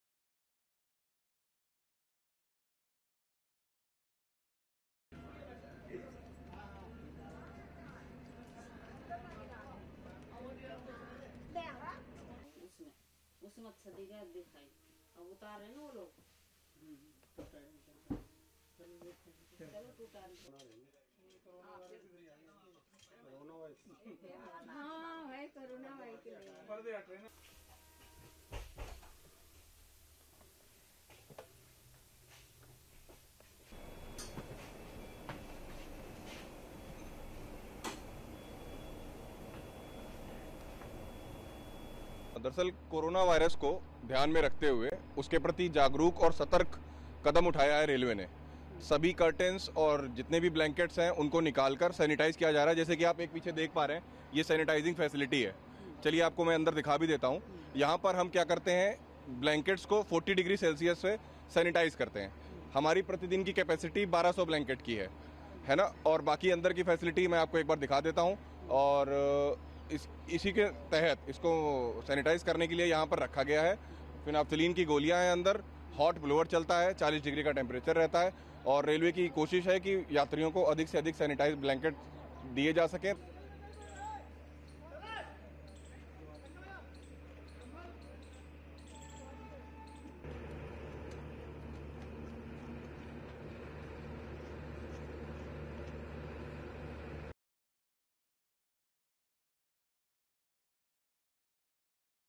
IRCTC takes precautionary measures to combat COVID-19
New Delhi :

 As fear of coronavirus continues to mount, Indian Railway Catering and Tourism Corporation (IRTC) is taking precautionary measures for the safety of passengers. Railway employees are thoroughly sanitising curtains and blankets.   “IRCTC is taking all necessary steps to combat coronavirus.  We are sanitising all curtains and blankets,” senior CDO, Jitendra told ANI.